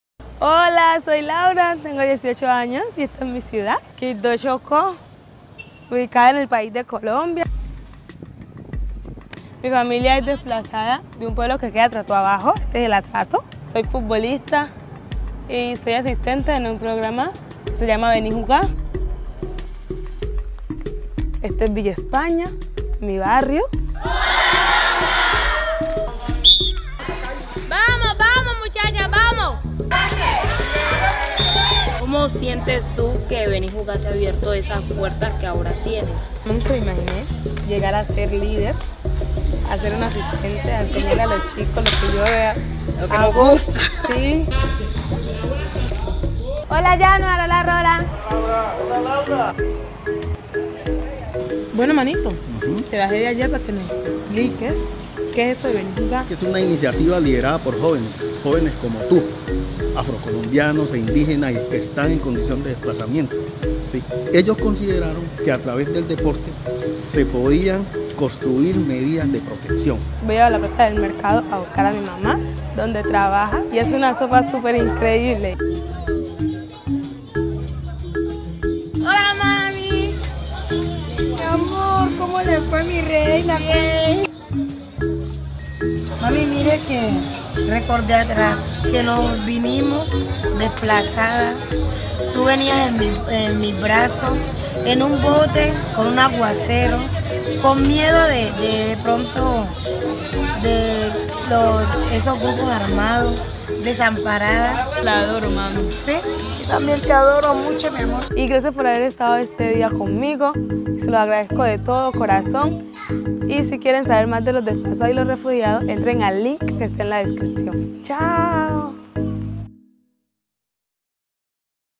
Laura, futebolista colombiana
Laura é uma deslocada interna colombiana que mostra como o futebol e sua família podem ajudar pessoas fugindo de conflitos a superar adversidades. Saiba mais